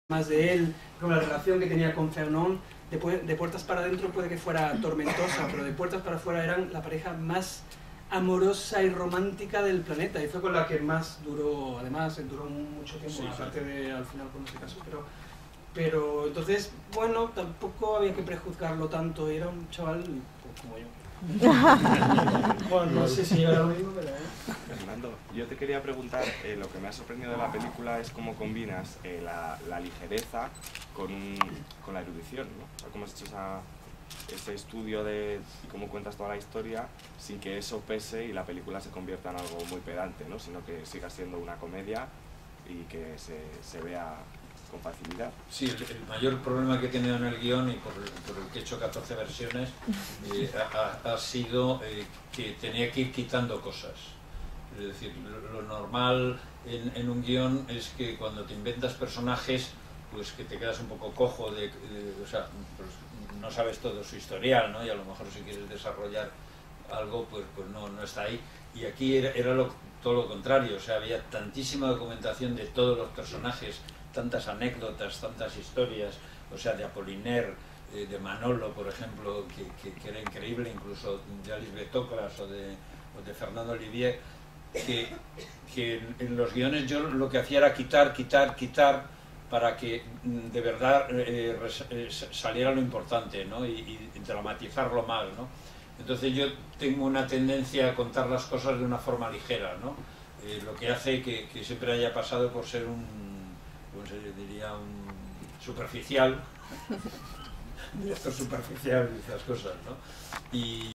La Banda Picasso: pase-rueda de prensa
Título: La Banda Picasso: pase-rueda de prensa

Descripción: Pase y rueda de prensa de `La Banda Picasso`, de Emilio Martínez Lázaro en los cines Princesa de Madrid el 21 enero de 2013, con la presencia de director, productor y actores.

Fecha: 23/01/2013

Materia: Ciencias de la Información

Submateria: Cine

Colaborador: Marinero Comunicación. Cristina Marinero. COMUNICACIÓN & RRPP

Formato: MPEG-2

Fuente: Complumedia-RTVDoc

Idioma: Español